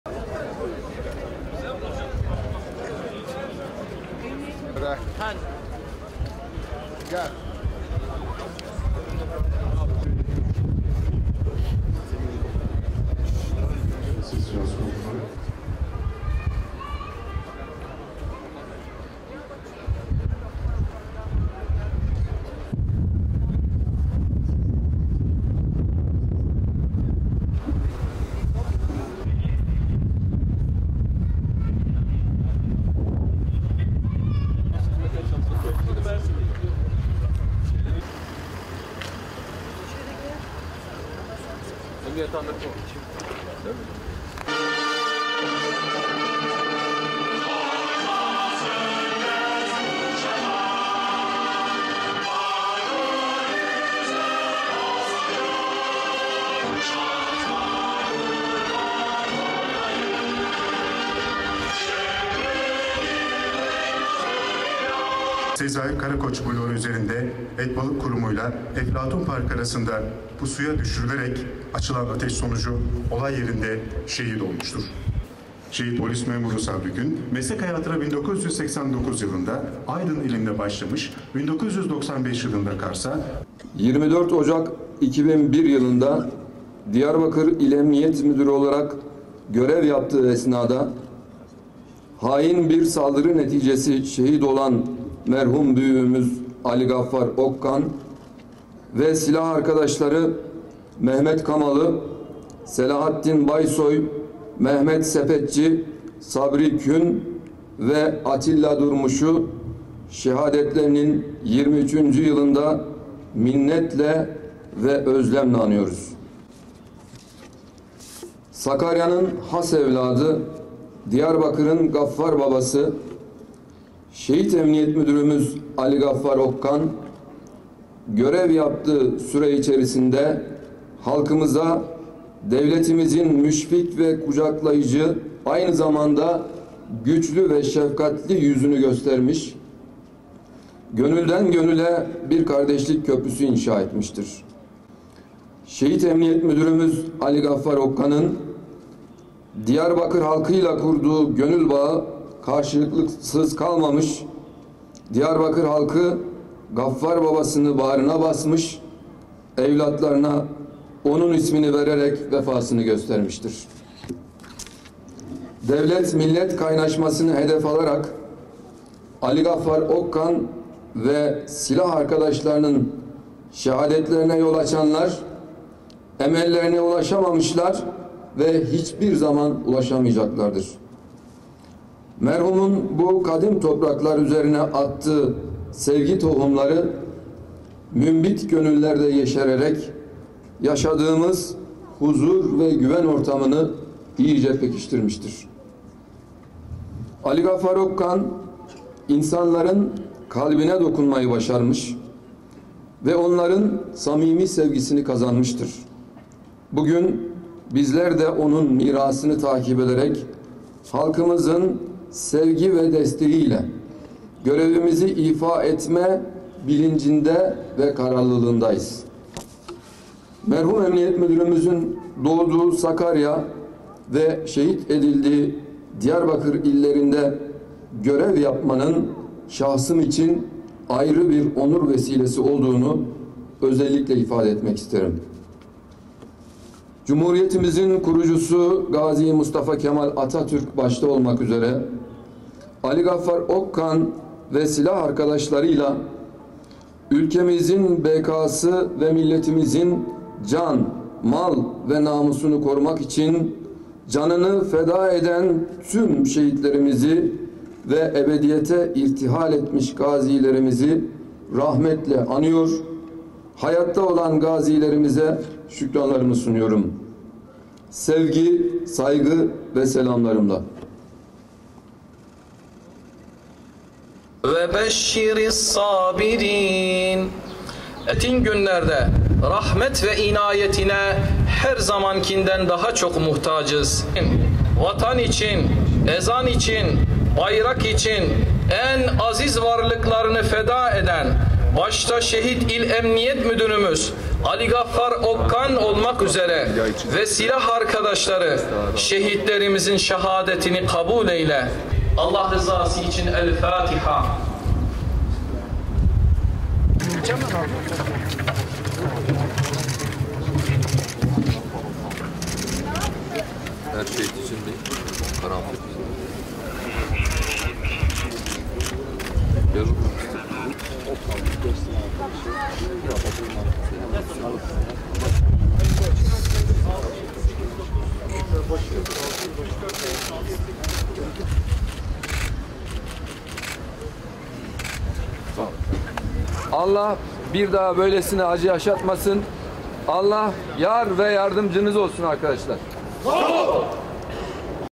Şehit edilen Gaffar Okkan ve 5 polis memuru törenle anıldı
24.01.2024
Diyarbakır'da 23 yıl önce silahlı saldırıda şehit edilen İl Emniyet Müdürü Ali Gaffar Okkan ve 5 polis memuru için anma töreni düzenlendi.